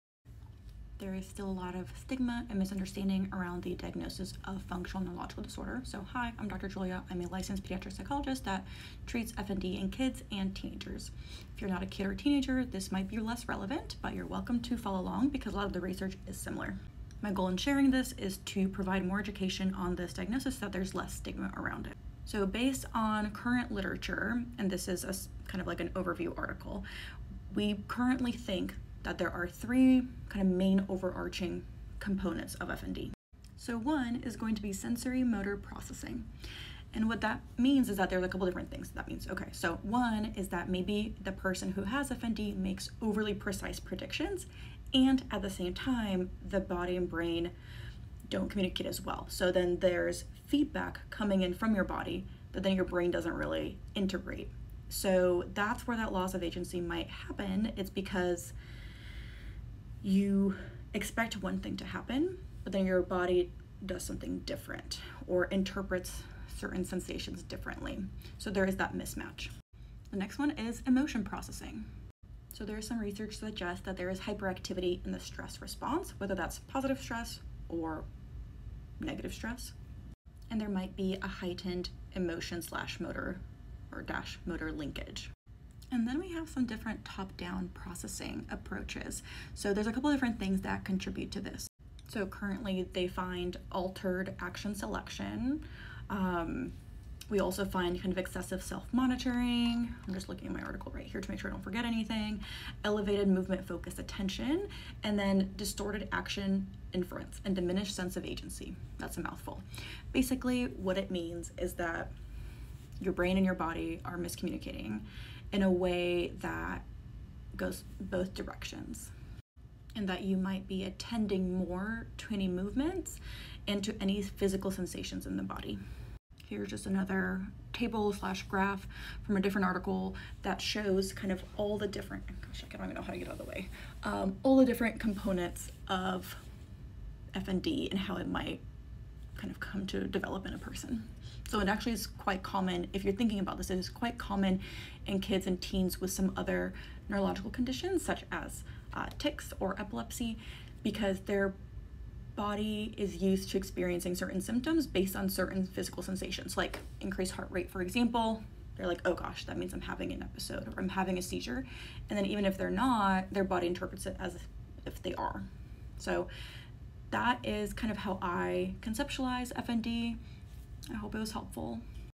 How I conceptualize FND development based on the recent research! Just like any other diagnosis,
How I conceptualize FND development based on the recent research! Just like any other diagnosis, it’s complex and there isn’t just one cause. Good news is that FND is treatable!